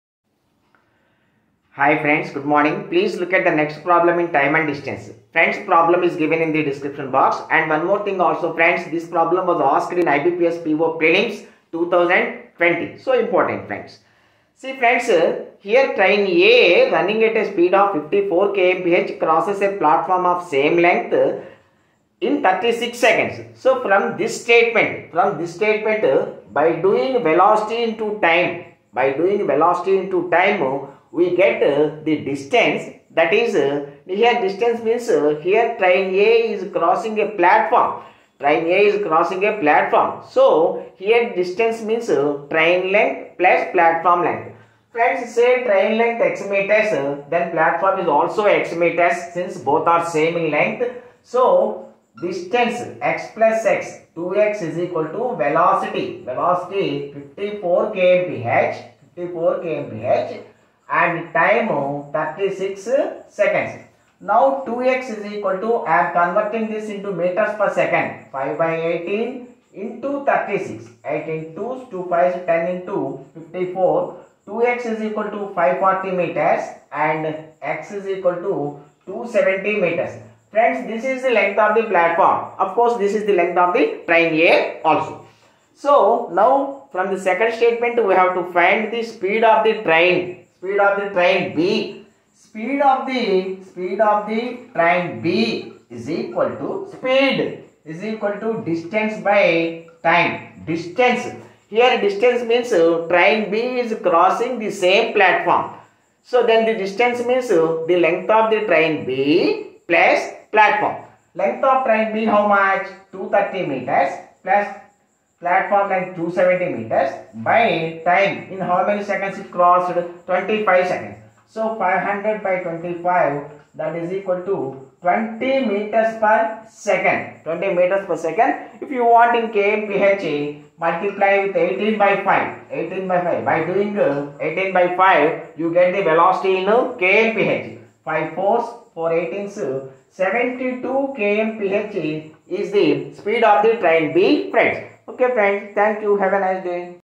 Train ‘A’ running at speed of 54 km/hr crosses a platform of length same as that of the train in 36
Train ‘A’ running at speed of 54 km/hr crosses a platform of length same as that of the train in 36 sec. If train B, which is 230 meters long crosses the same platform in 25 sec, then find speed of train B (in km/hr)?